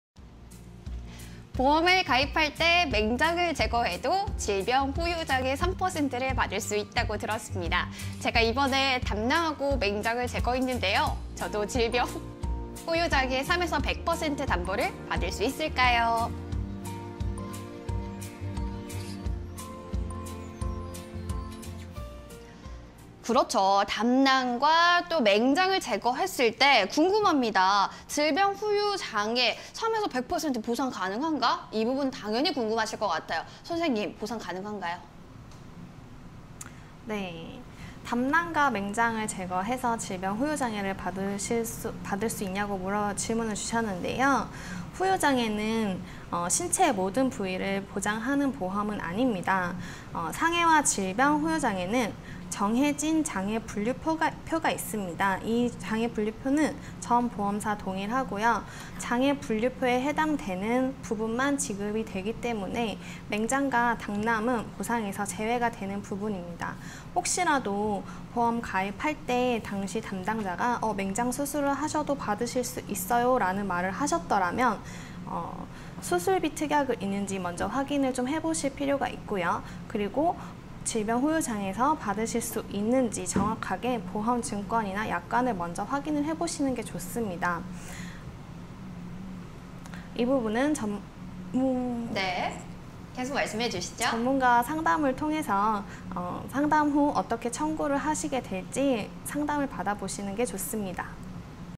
담낭, 맹장 제거 시 질병후유장해 보상 가능할까?_보험 Q&A (20210618)
담낭, 맹장 제거 시 질병후유장해 보상 가능할까?